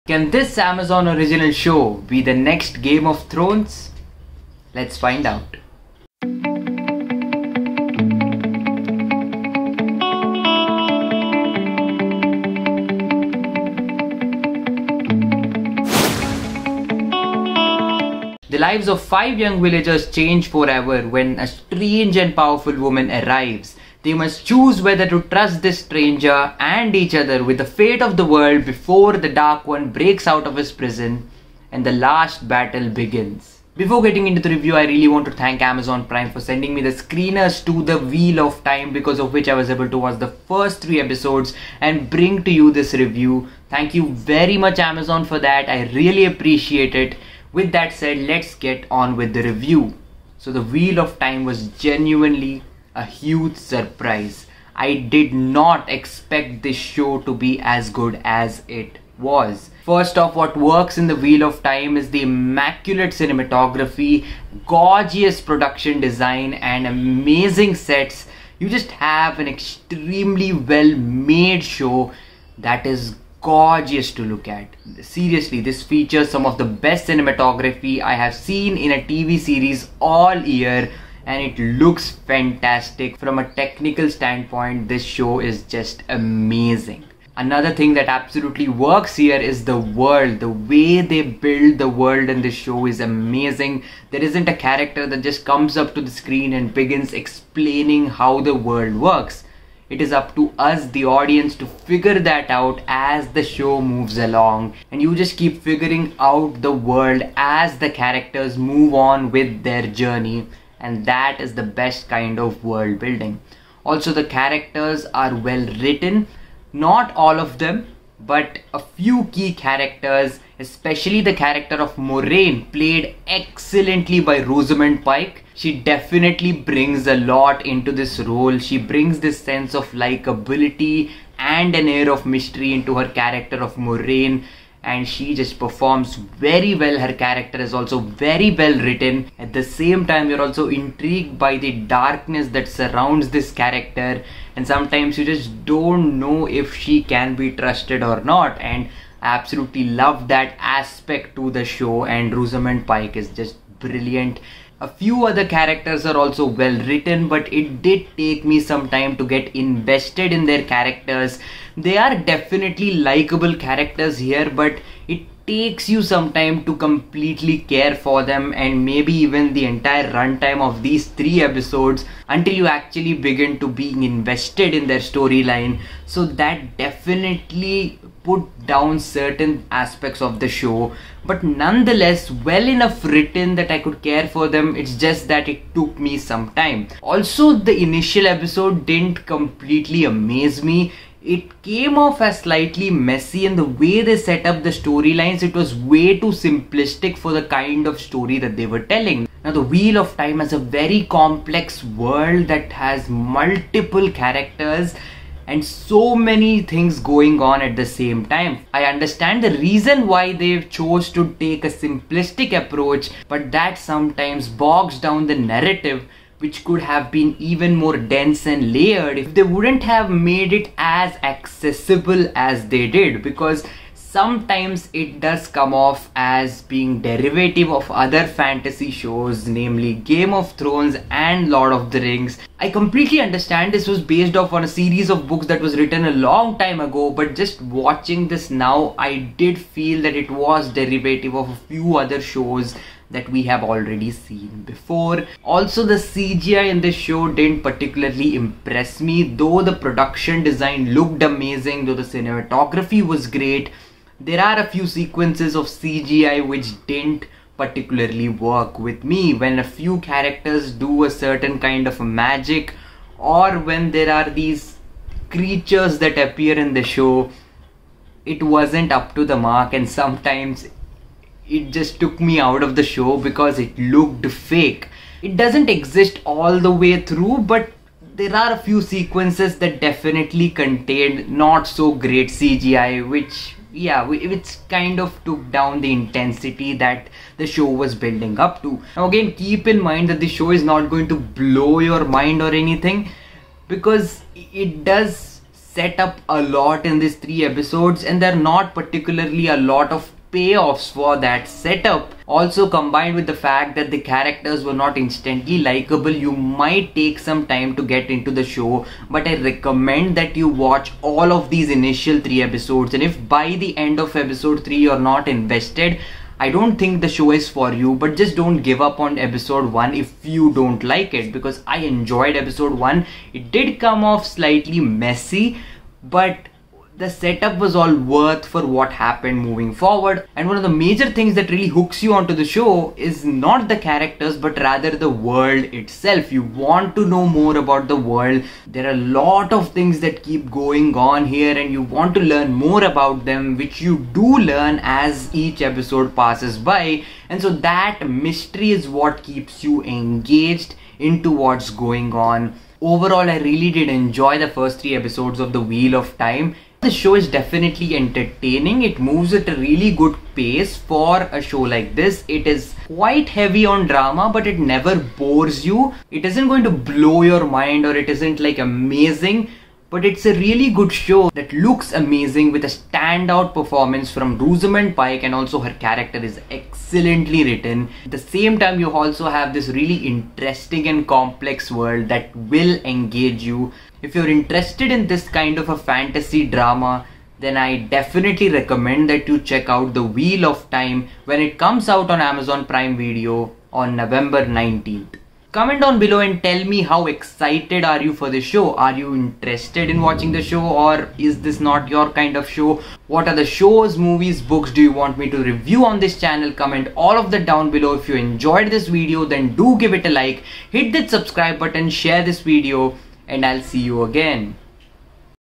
THE WHEEL OF TIME Series REACTION & REVIEW | Episodes 1-3
My Spoiler Free The Wheel Of Time Review, for the first three episodes of Season 1. What did you think about The Wheel Of Time? What is your The Wheel Of Time Reaction and Review? Tell me if you are excited for The Wheel Of Time Reaction in the Comments Below.

PLOT
The Wheel of Time is one of the most popular and enduring fantasy series of all time, with more than 90 million books sold. Set in a sprawling, epic world where magic exists and only certain women are allowed to access it, the story follows Moiraine (Rosamund Pike), a member of the incredibly powerful all-female organization called the Aes Sedai, as she arrives in the small town of Two Rivers. There, she embarks on a dangerous, world-spanning journey with five young men and women, one of whom is prophesied to be the Dragon Reborn, who will either save or destroy humanity.

DIRECTED BY
Uta Briesewitz, Wayne Che Yip, Salli Richardson-Whitfield, Ciaran Donnelly

PRODUCED BY
Amazon Studios and Sony Pictures Television

CAST
Rosamund Pike – Moiraine Damodred
Daniel Henney – Lan Mondragoran
Josha Stradowski – Rand al’Thor
Madeleine Madden – Egwene al’Vere
Marcus Rutherford – Perrin Aybara
Zoë Robins – Nynaeve al'Meara
Barney Harris – Mat Cauthon

EXECUTIVE PRODUCERS
Rafe Judkins, Larry Mondragon, Rick Selvage, Mike Weber, Ted Field, Darren
Lemke, Marigo Kehoe, and Uta Briesewitz

DEVELOPED BY
Rafe Judkins